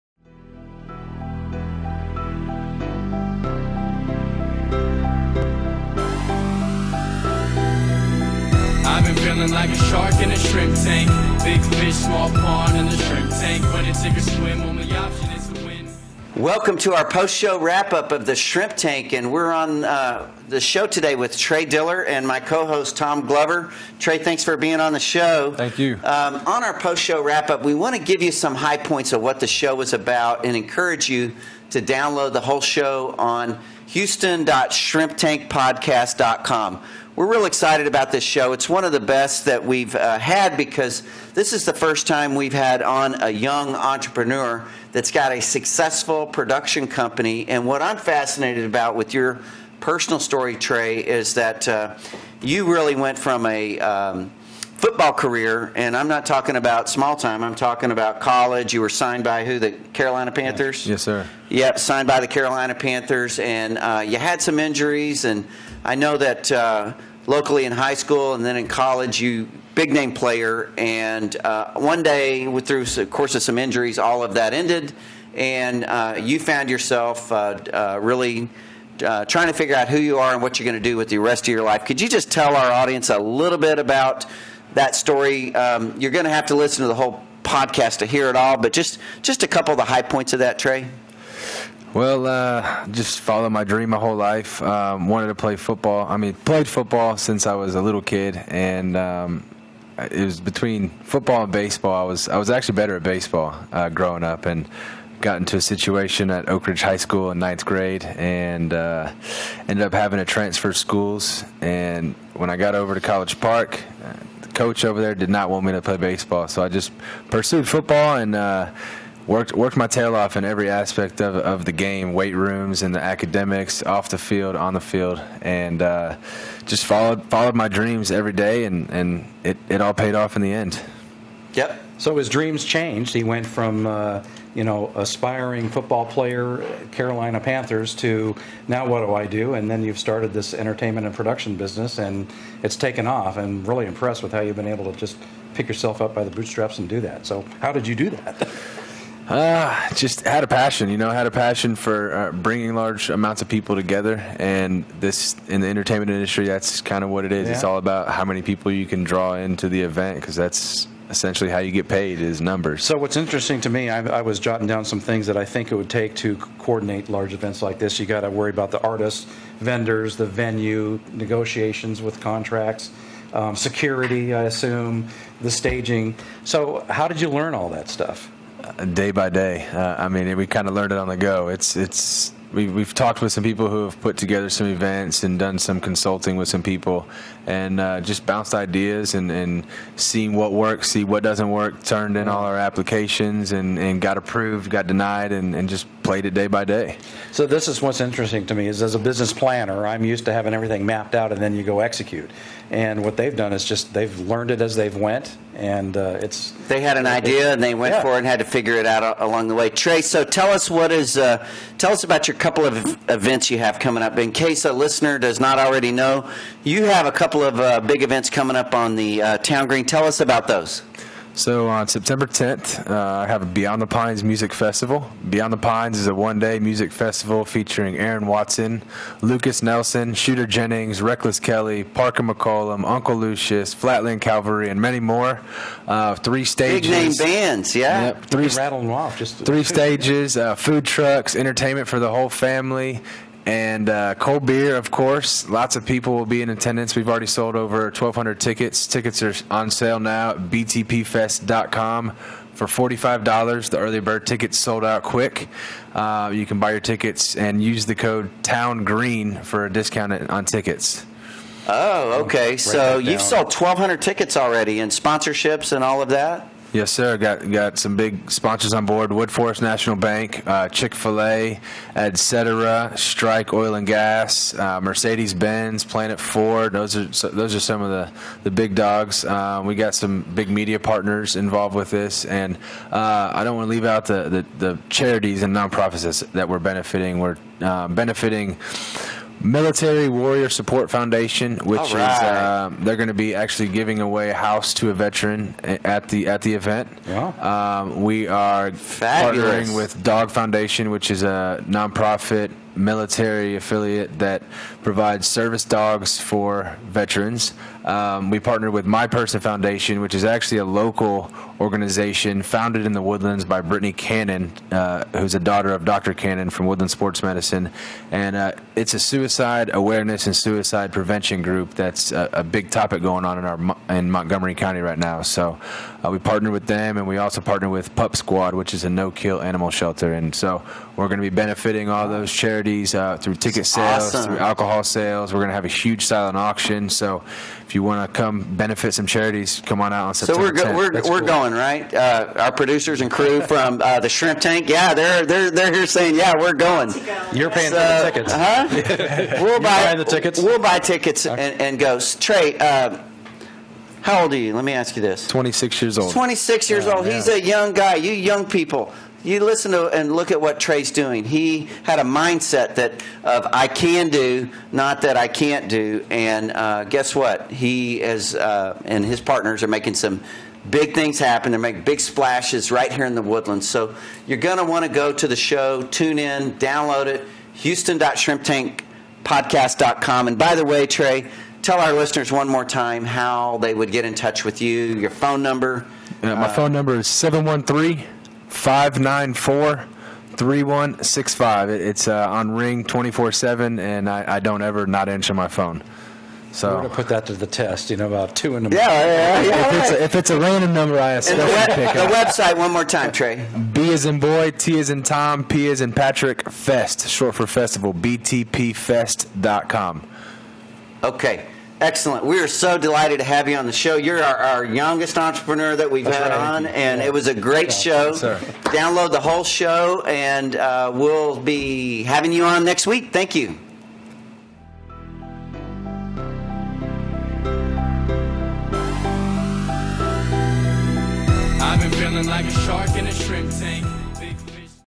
Houston Shrimp Tank Episode 12 - Trey Diller - JACT Entertainment & Production
Trey Diller, an entrepreneur based in The Woodlands, started out pursuing a sports career after college. After 2 National Championships at Sam Houston State University, Trey signed with the Carolina Panthers. Unfortunately, injuries plagued him. More callouts with teams resulted but more injuries caused him to hang up his cleats.